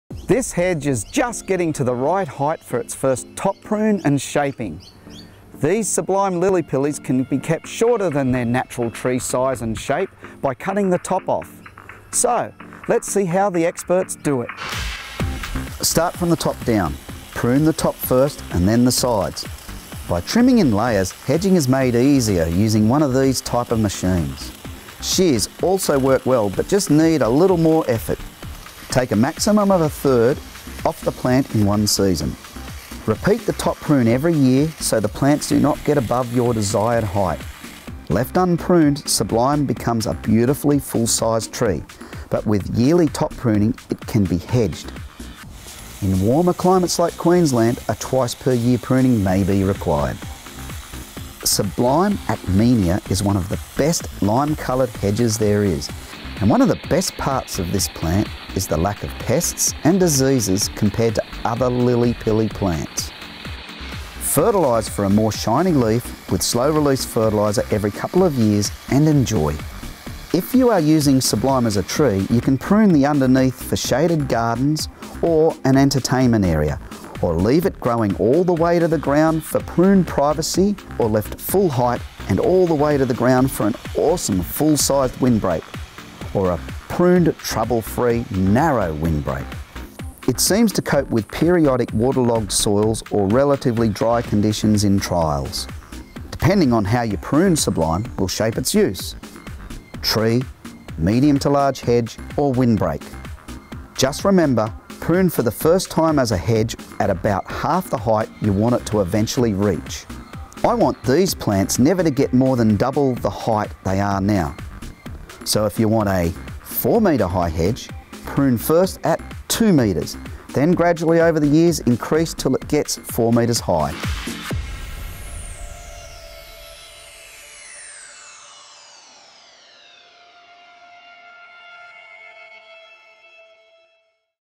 Maintaining Sublime™ Acmena Lilly Pilly
Sublime™ Acmena Lilly Pilly is a lovely mid-sized tree with refreshing lime new growth and dense foliage to the ground, great as a hedging or screening tree. Masses of white fluffy flowers appear in summer and it rarely produces berries. For a tidy hedge, prune yearly or for a natural shape prune every 2-3 years.